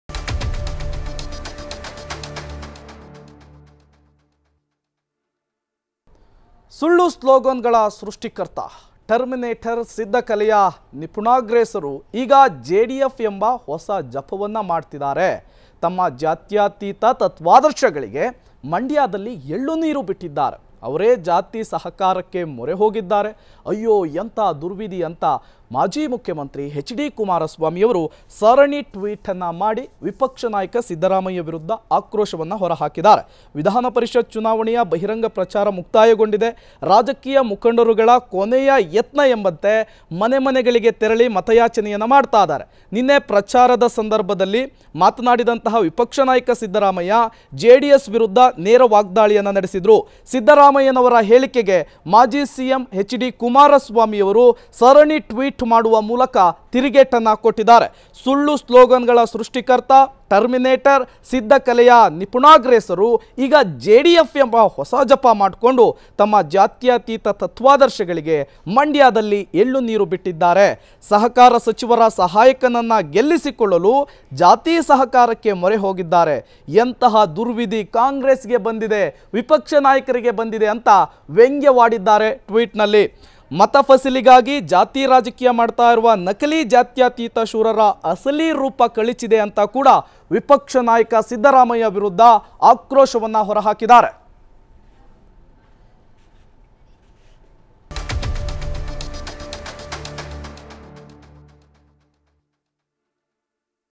ವಿನಾಶಕಾಲೇ ವಿಪರೀತ ಸುಳ್ಳು.. ಹೆಚ್‌ಡಿಕೆ ಟ್ವೀಟ್‌
ವಿನಾಶಕಾಲೇ ವಿಪರೀತ ಸುಳ್ಳು.. ಆ ಸುಳ್ಳುಗಳೇ ಅವರನ್ನು ಸುಡುತ್ತದೆಆಟ ಈಗ ಆರಂಭ ಎಂದು ಟ್ವೀಟ್‌ ಮಾಡಿ ಸಿದ್ದುಗೆ ಗುದ್ದು ನೀಡಿದ ಹೆಚ್‌ಡಿಕೆ